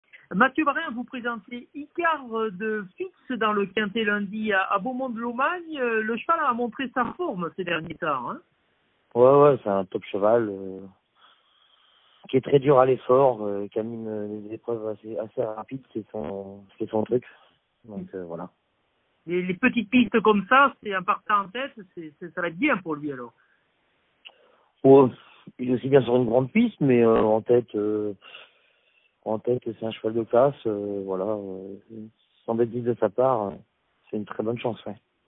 MATTHIEU VARIN | ENTRAÎNEUR DE ICARE DE PHYT'S | QUINTÉ DU 9 SEPTEMBRE À BEAUMONT-DE-LOMAGNE
MATTHIEU VARIN, ENTRAÎNEUR DE ICARE DE PHYT'S (1) nous parle de ses chances dans le quinté du lundi 9 septembre 2024, le Grand Prix Jean Dumouch sur l’hippodrome de Beaumont-de-Lomagne.